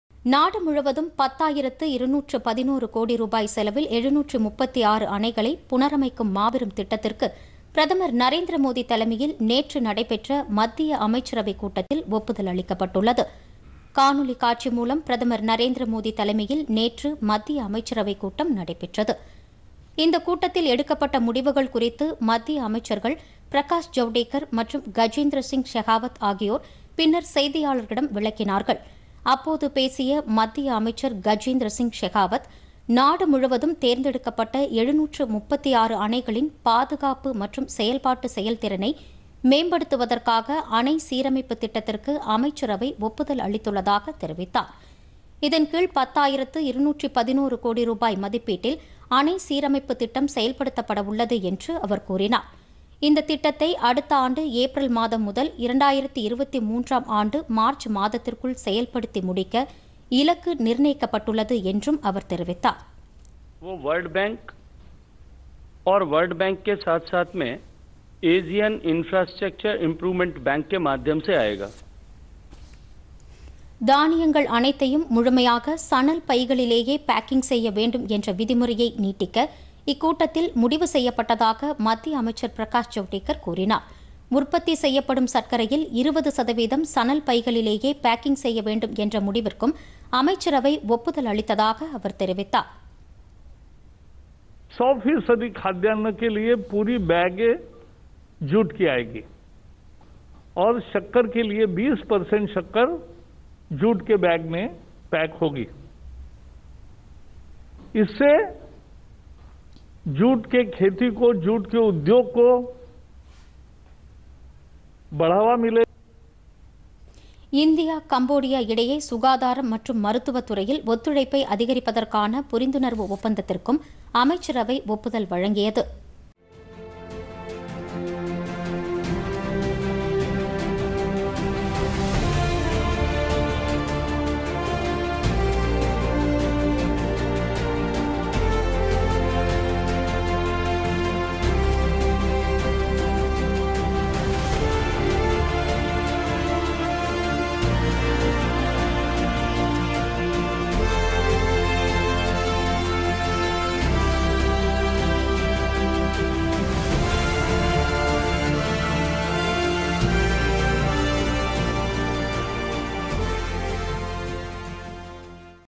736 அணைகளை புனரமைக்கும் மாபெரும் திட்டம்PodhigaiTamilNews
நாடு முழுவதும் 10 ஆயிரத்து 211 கோடி  ரூபாய் செலவில்  736 அணைகளை புனரமைக்கும் மாபெரும் திட்டம்-
**பிரதமர் நரேந்திர மோதி தலைமையிலான மத்திய அமைச்சரவைக் கூட்டத்தில் ஒப்புதல்.